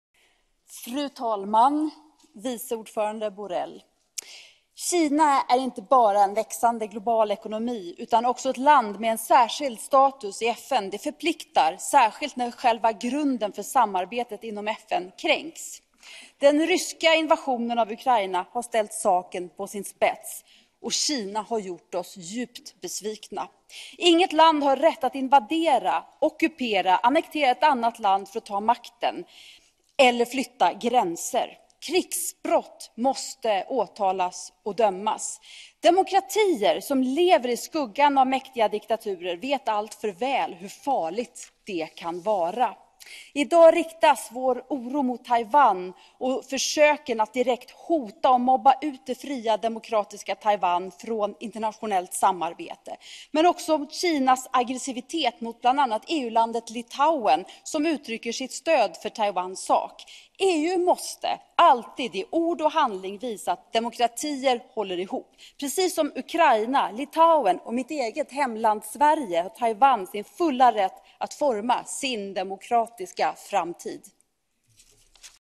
Karin Karlsbro 05 April 2022 plenary speech on EU China Summit
Karin Karlsbro 05 April 2022 plenary speech on Outcome of the EU-China Summit (1 April 2022) - Council and Commission statements